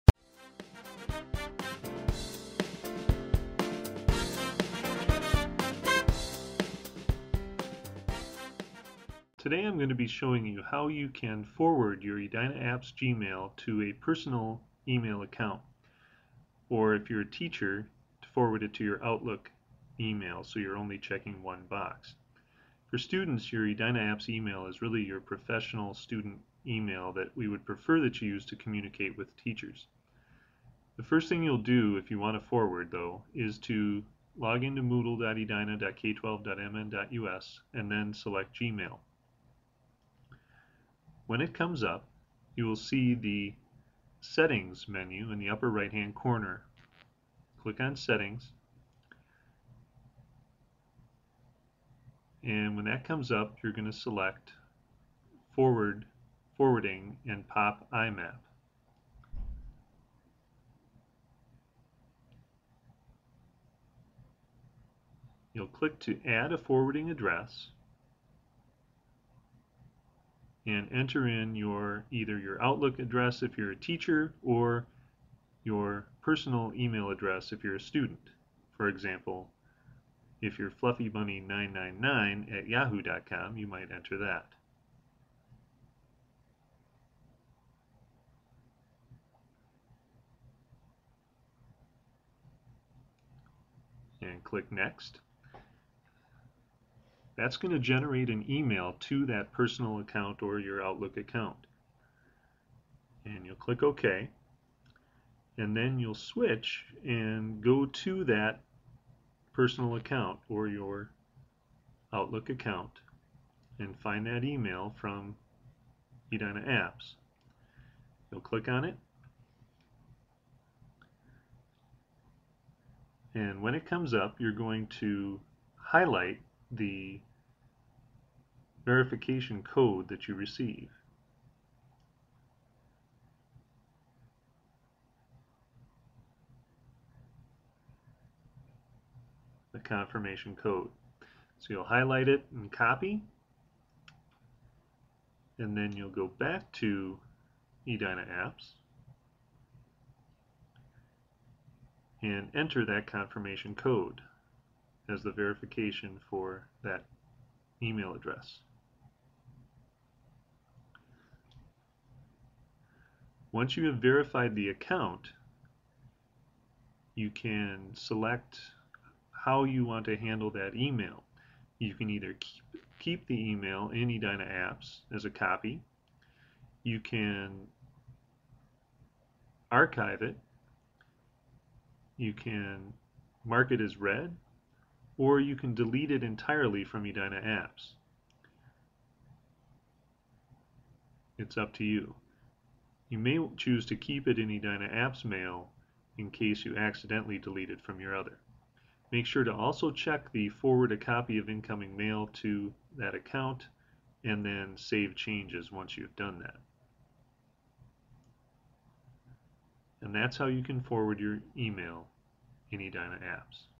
Forwarding Edina Apps E-mail
This video explains how to forward your e-mail in Edina Apps.